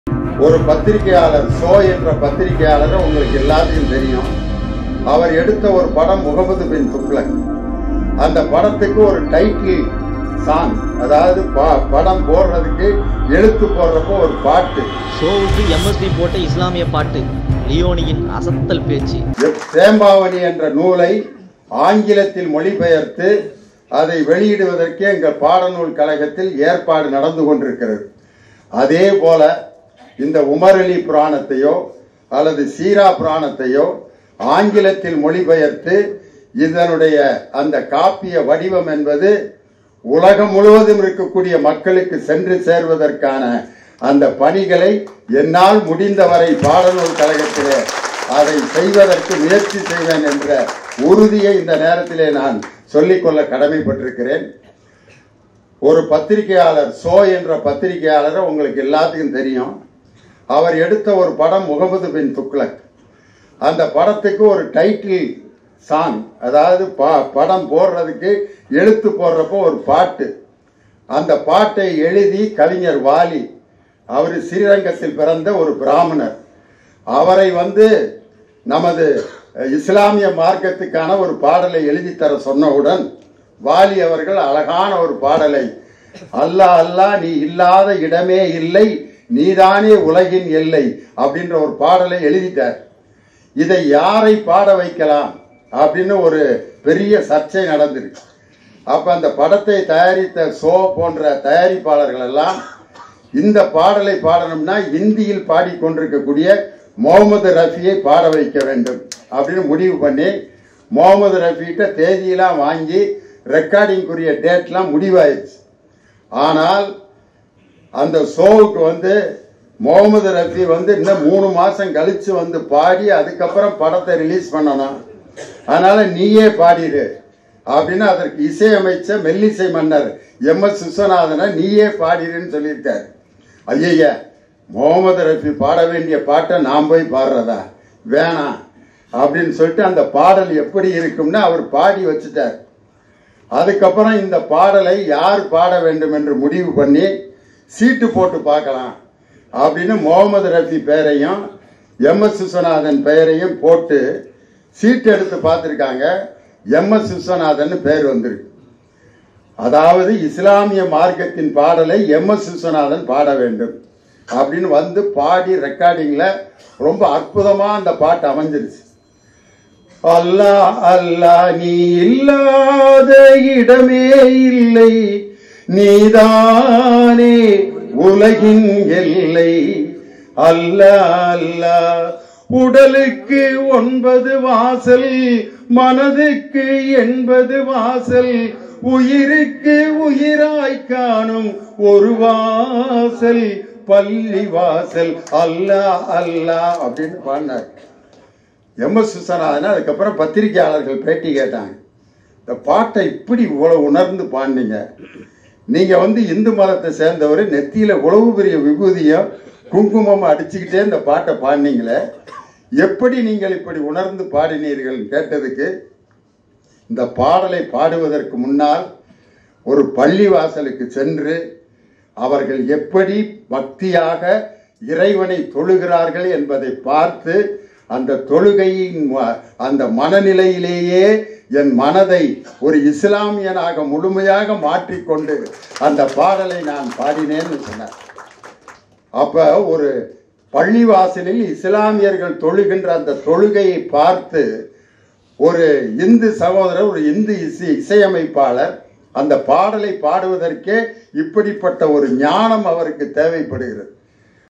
சோவுக்கு MSV போட்ட இஸ்லாமிய பாட்டு! லியோனியின் அசத்தல் பேச்சு | Dindigul I Leoni speech
சோவுக்கு MSV போட்ட இஸ்லாமிய பாட்டு! லியோனியின் அசத்தல் பேச்சு | Dindigul I leoni speech | I leoni Speech